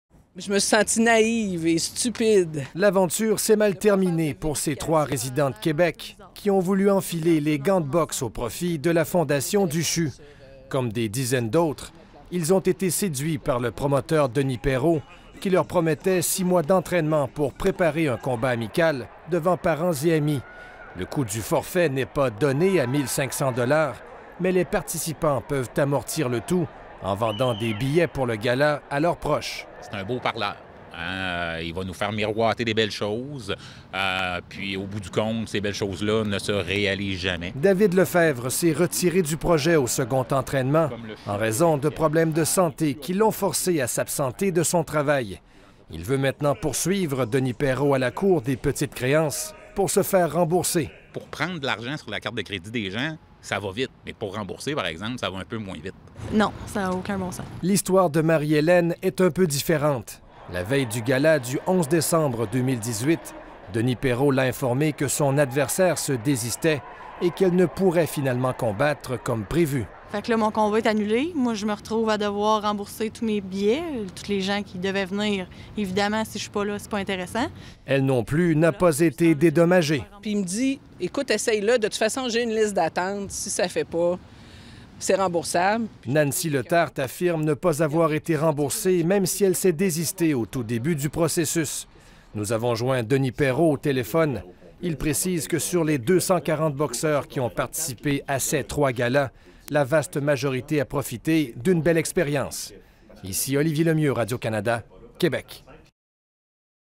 Des boxeurs du dimanche floués
Des personnes qui s’étaient inscrites pour combattre dans un gala de boxe au profit du CHU de Québec affirment ne jamais avoir été remboursées par le promoteur, bien qu'elles n'aient jamais combattu.